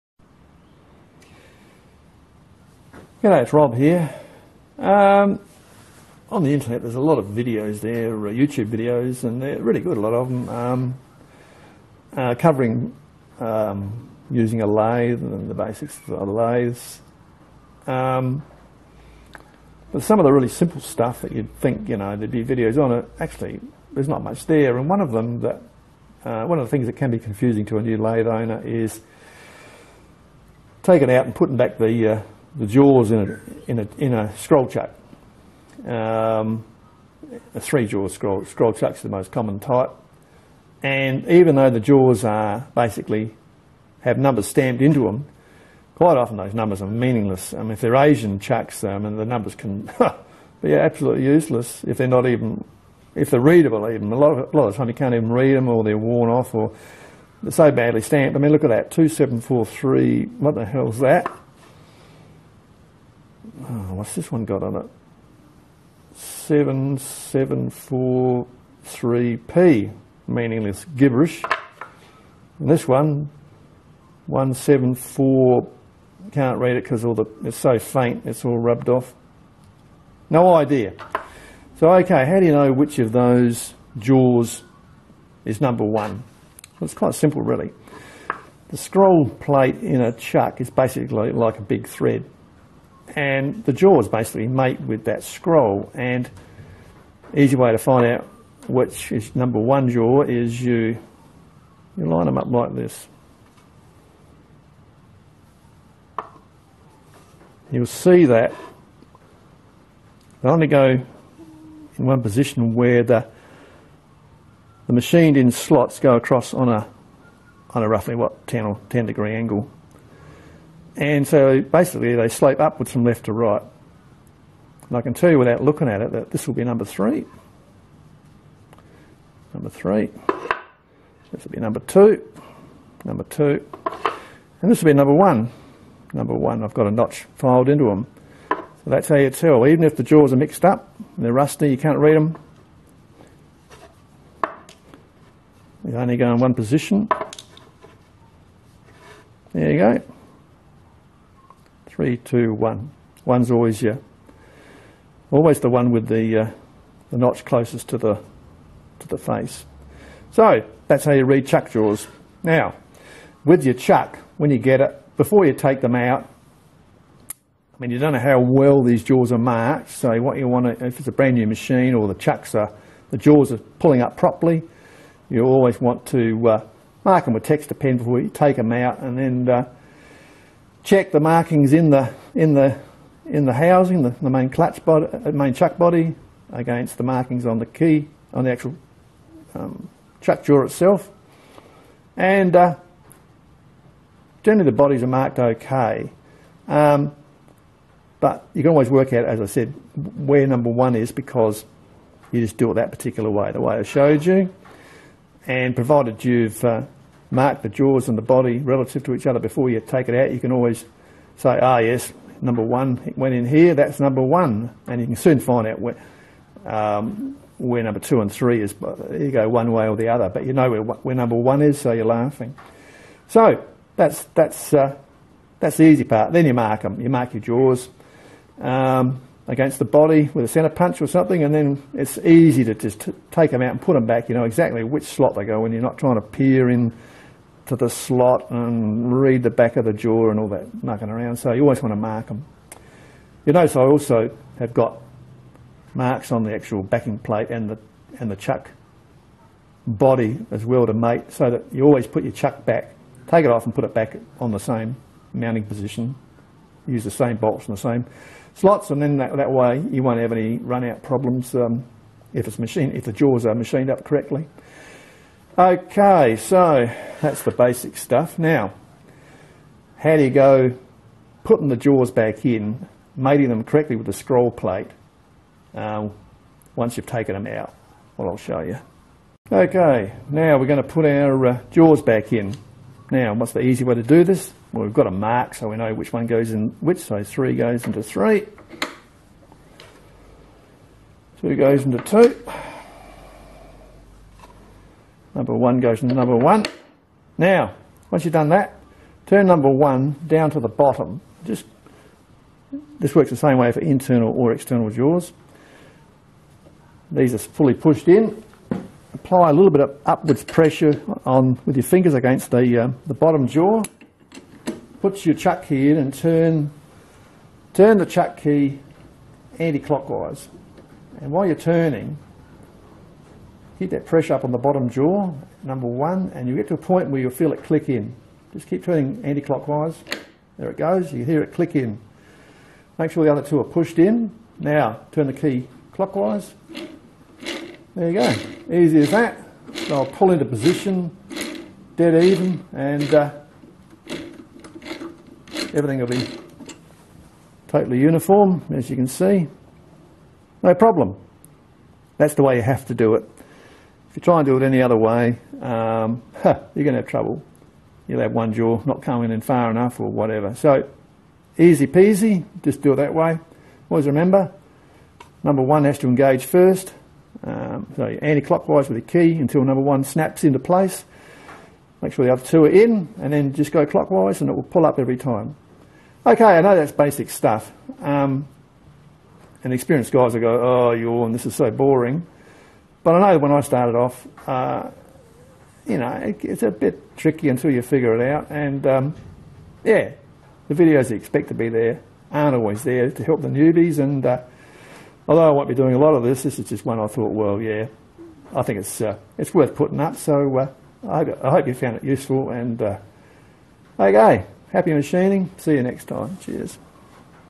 How to change the chuck jaws on a metal lathe
One for beginners.  Shows how to number and correctly fit scroll chuck jaws.